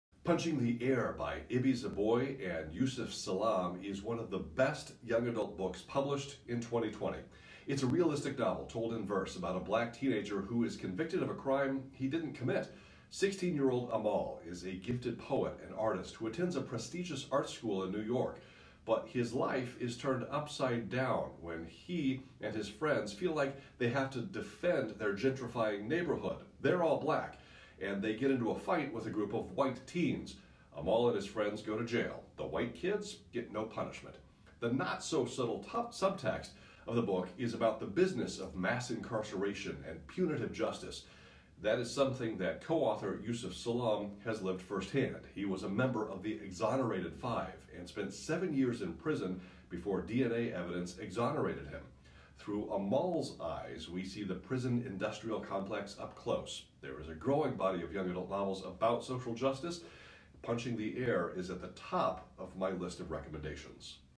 Punching the Air by Ibi Zoboi and Yusef Salaam Spoiler Free Book Talk
A high school librarian gives a spoiler-free book talk about the young adult social justice novel Punching the Air by Ibi Zoboi and Yusef Salaam.